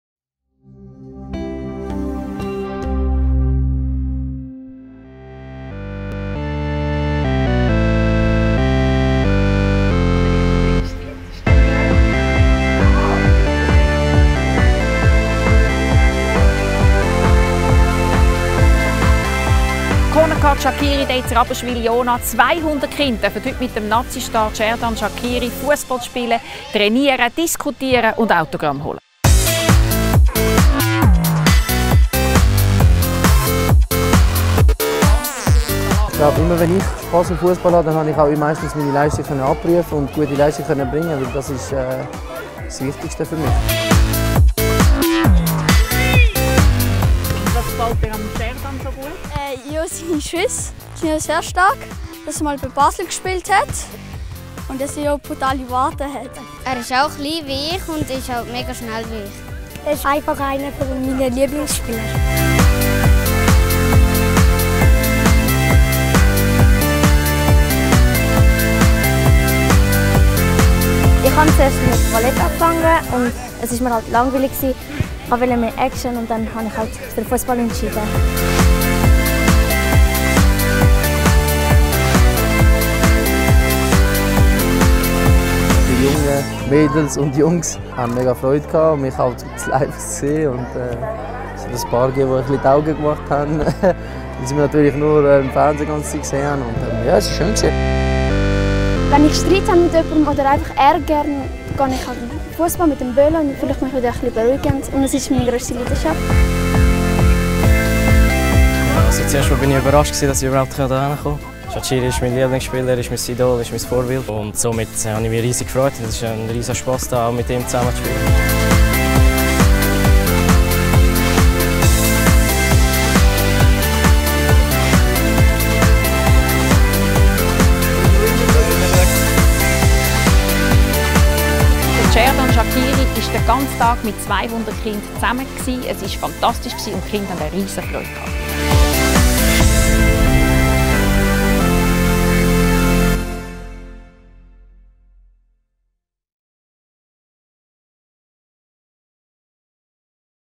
Cornèrcard Shaqiri Day 2017 (long version)
Anpfiff zum 1. Cornèrcard Shaqiri Day! Der Andrang war gewaltig, aber nur die Gewinner unseres Cornèrcard Emotions Wettbewerbs konnten ihr Idol Xherdan Shaqiri bei unserem Event persönlich treffen. Ein Tag voller Fussballtricks, Selfies, spontanen Matches und glücklichen Kinderaugen liegt hinter uns. Die schönsten Momente haben wir in diesem Video für euch festgehalten.